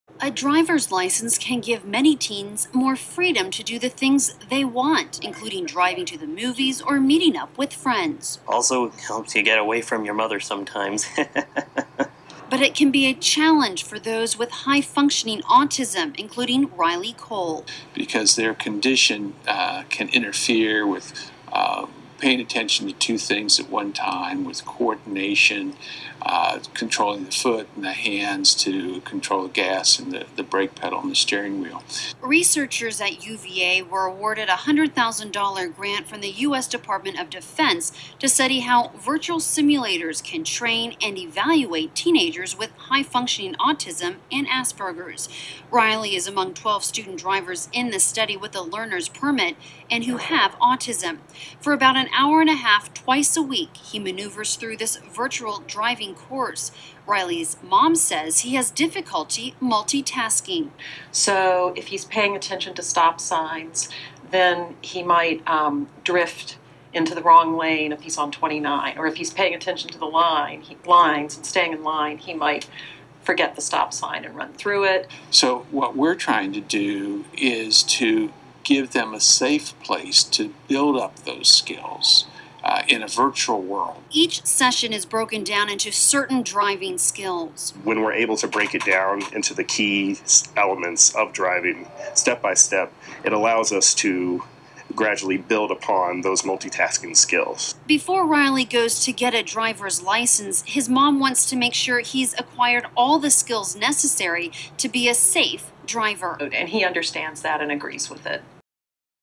Youth-Nex: Teens with Autism Drive in Virtual World
Youth-Nex: Teens with high-functioning autism and Asperger's learn to drive in virtual world